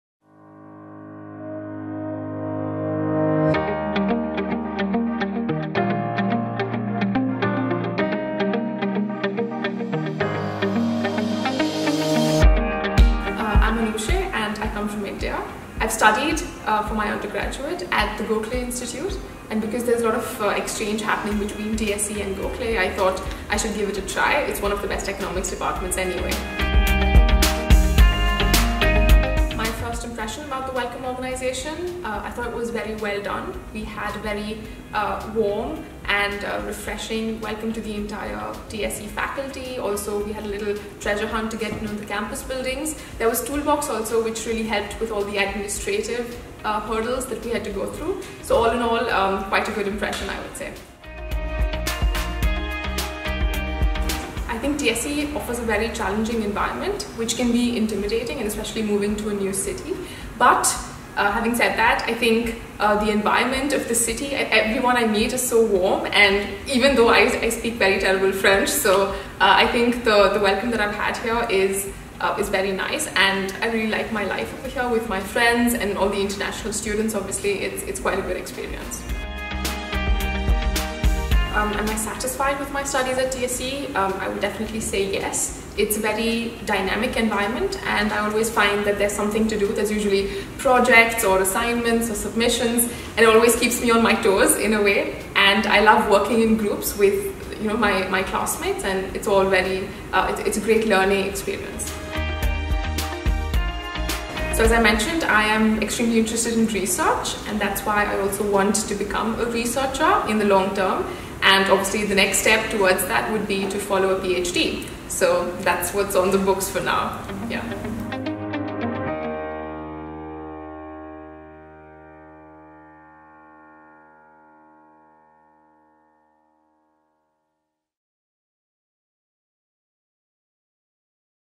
Student Spotlights - Anoosheh: Master in Economics - Toulouse School of Economics
Tu cherches ton orientation dans le domaine de l'économie, de l'environnement et des techniques quantitatives ?
Are you looking for your orientation in the field of economics, environment and quantitative techniques?
Anoosheh, étudiante en Master 1 in Economics nous partage son retour.

0:00 Intro
0:12 Where do you come from and why did you choose TSE?
0:26 Your first impressions and opinion about the welcome organization ?
0:54 How do you feel at TSE, was it easy to adapt when you arrived in Toulouse??
1:20 Are you satisfied of your studies at TSE and why?
1:43 What are your future career plans?
2:00 Outro